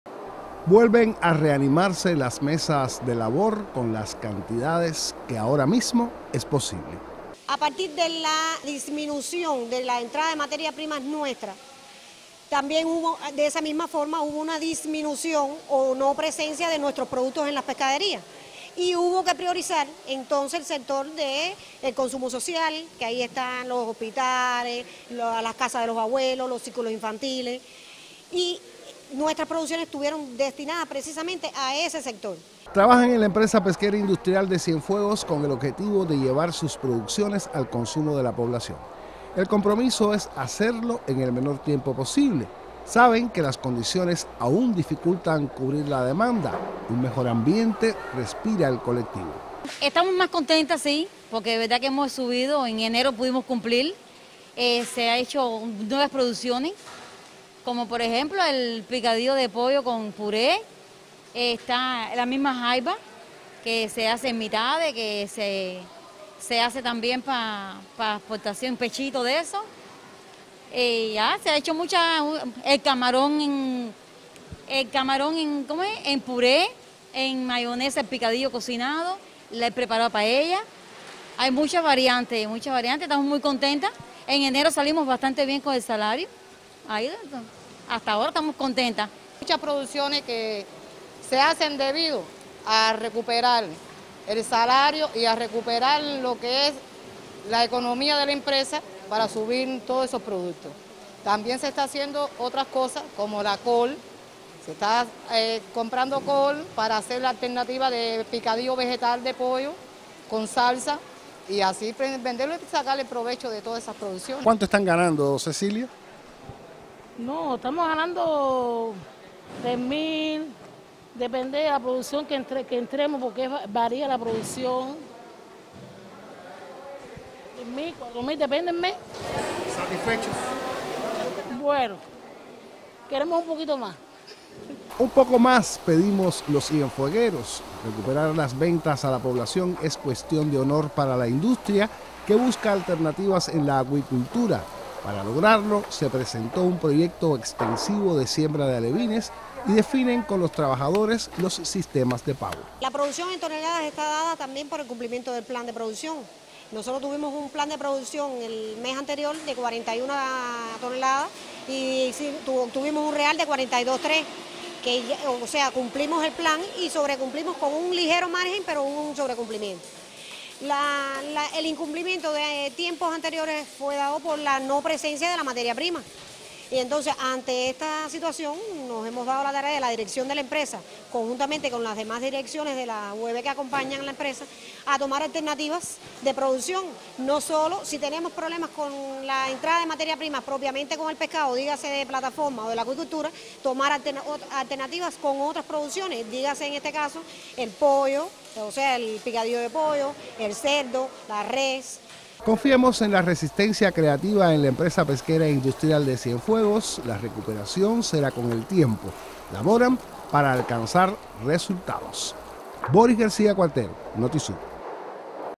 Diversifica producciones Empresa Pesquera Industrial de Cienfuegos
La Empresa Pesquera Industrial de Cienfuegos diversifica sus producciones con el objetivo de ofrecer más alimentos a sus clientes y recuperar las pérdidas que lastra la economía de la entidad. Se trabaja con el propósito de abastecer en el menor tiempo posible las pescaderías del territorio.